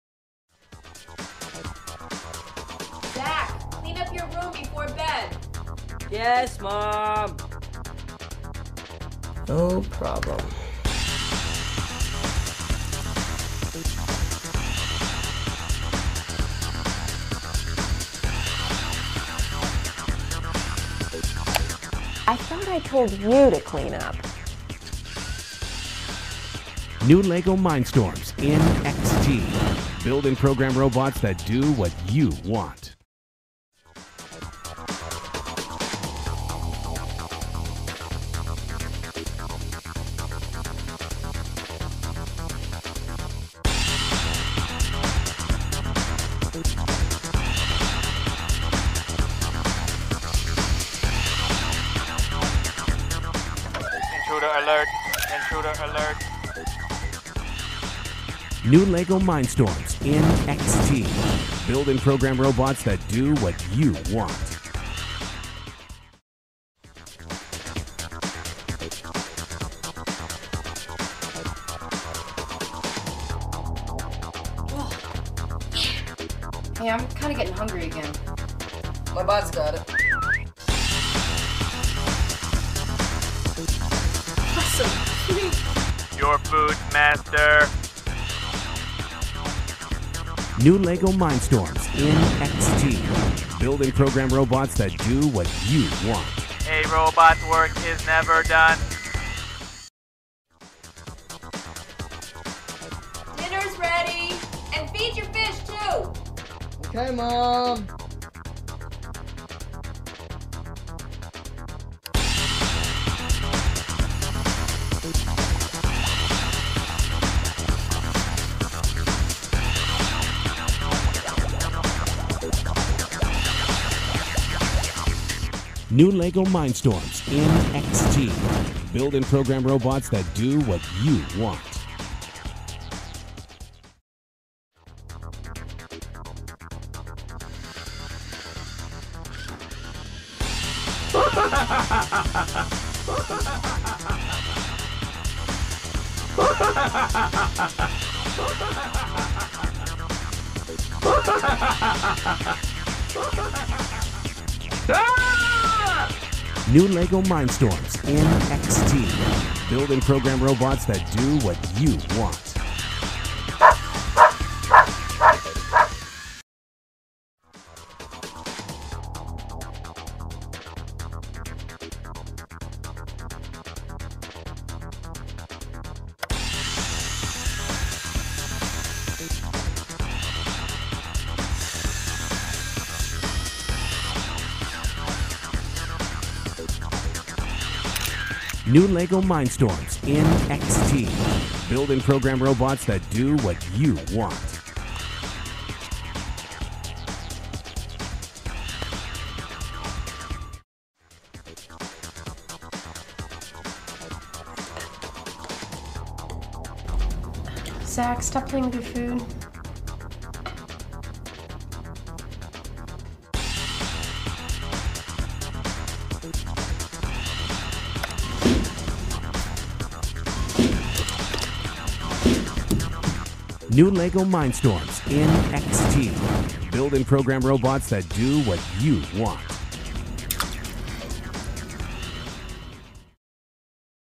8547 Лего Майндстормс NXT 2.0
Новые модели, расширенные возможности настроек программирования и полностью обновленные технологии! В MINDSTORMS NXT 2.0 представлено многообразие систем конструирования LEGO и полностью обновленное програмное обеспечение. LEGO MINDSTORMS NXT снова с нами, но еще лучше, чем когда-либо! Lego 8547 сочетает в себе неограниченную гибкость конструктора Lego, интеллект современных микропроцессоров, и возможности удобного программного обеспечения